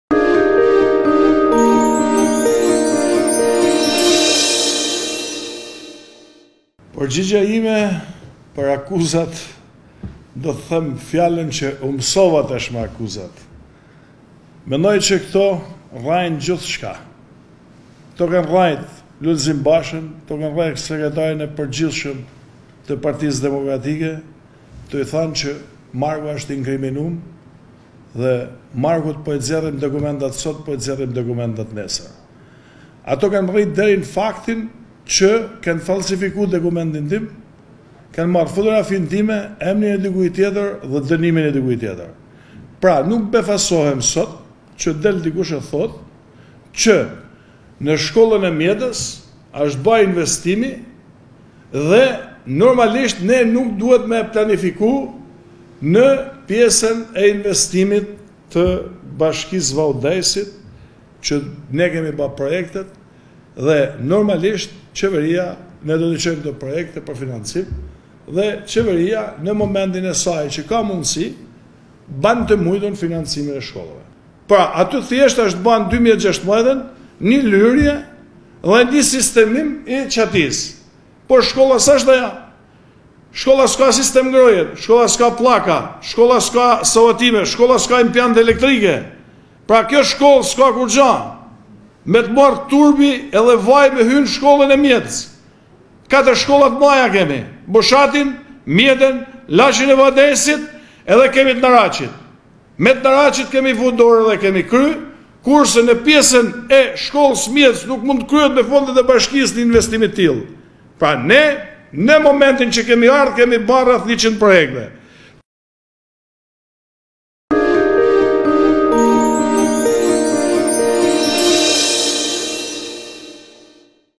ShkodraWeb I Kryebashkiaku i Vau- Dejësit Mark Babani pergjigje akuzave të PD për abuzime me fondet
Pak orë pasi PD Vau Dejës e akuzoi për abuzime me fondet për rikonstruksionin e shkollave, Mark Babani ka dalë para mediave për të dhënë versionin e tij. Ai ka ftuar mediat në Bashkinë e Vau- Dejësit të cilën e kryeson pas zgjedhjeve të 30 qershorit 2019.

Babani fillimisht, ka folur lidhur me pozicionin e tij lidhur me denoncimin që ka bërë PD për të kaluarën e tij të errët, e cila sipas opozitës duhet ti shkaktojë atij ndërprerjen e mandatit.